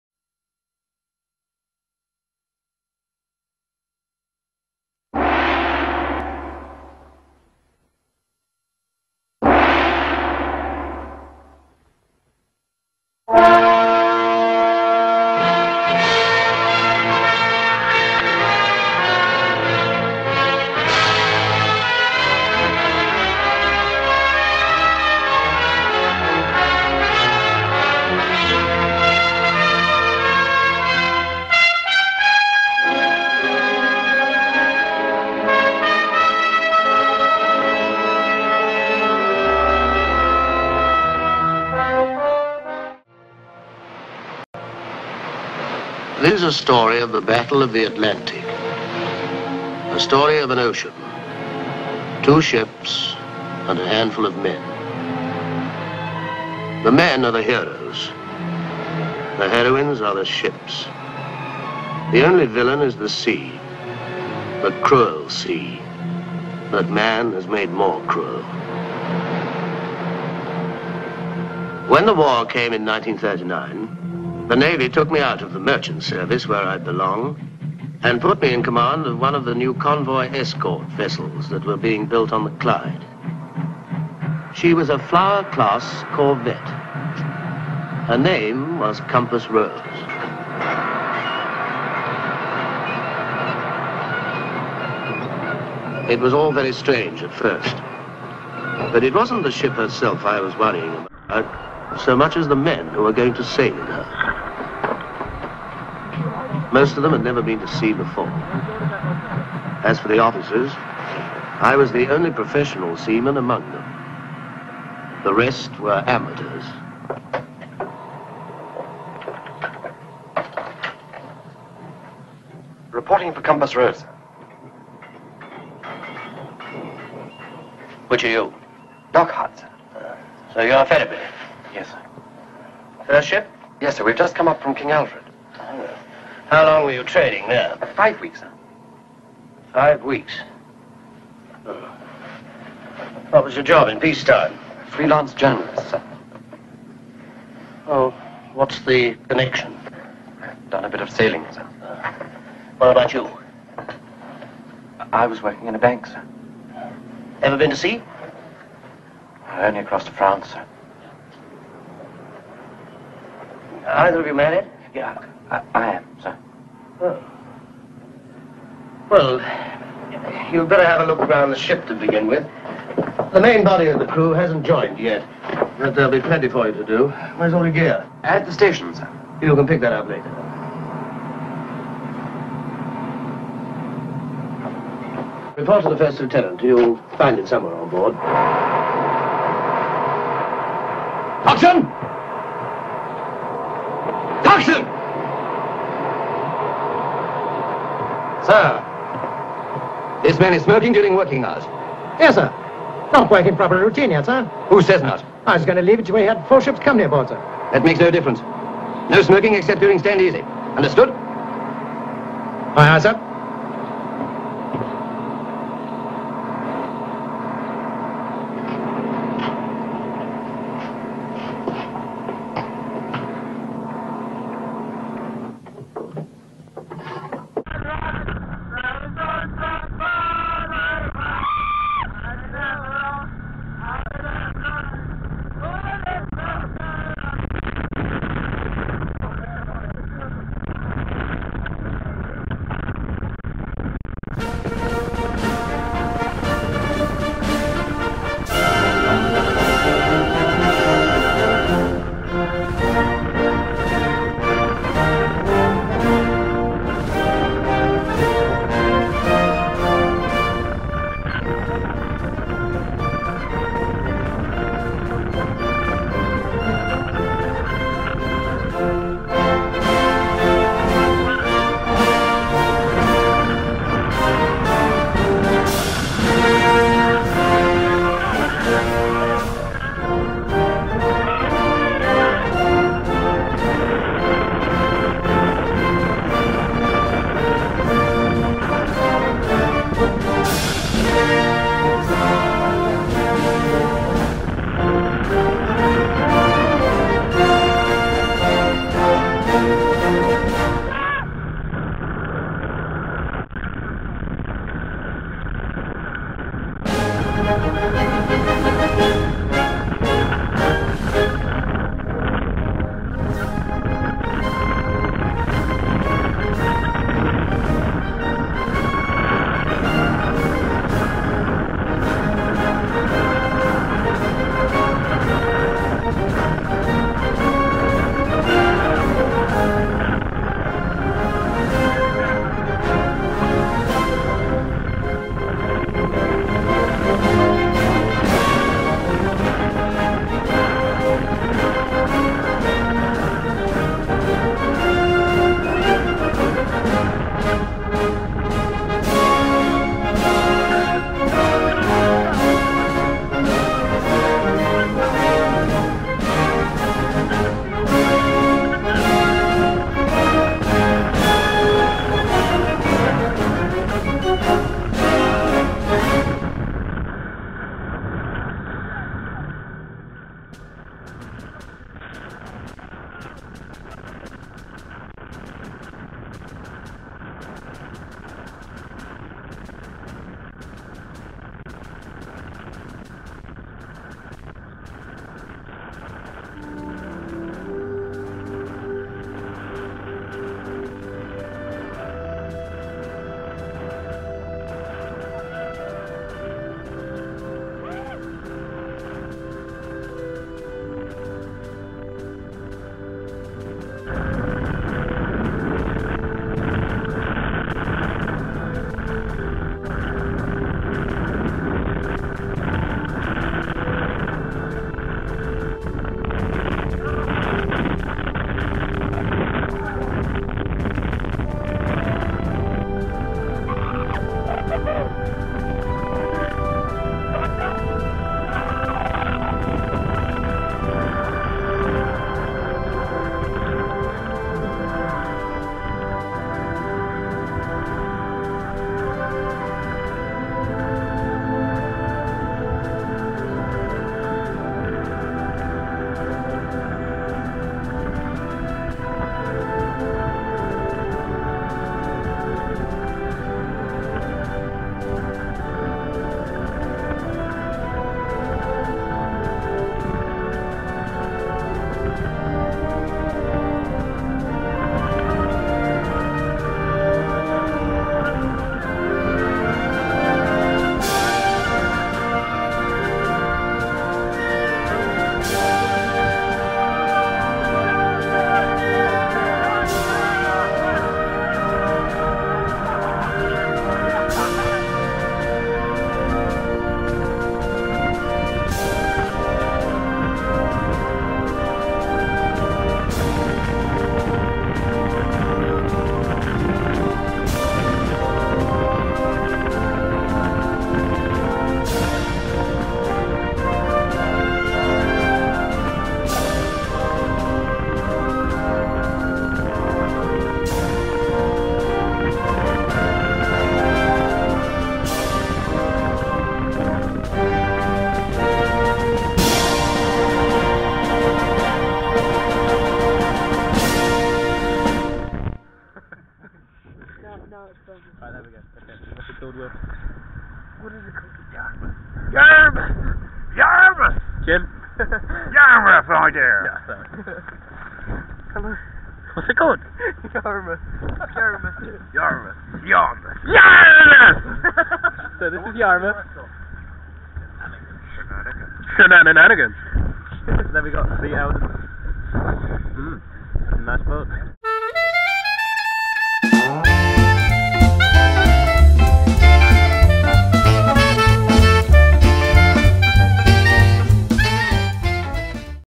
Haslemere RUFC Under 15's; The Cruel Sea
Haslemere RUFC Under 15's 2010 Viking Tour take to the high seas. 10 mins of what is, pretty ropey action cam footage, of rib action on the trip over to the Isle of Wight. No doubt those present will enjoy more than anyone else!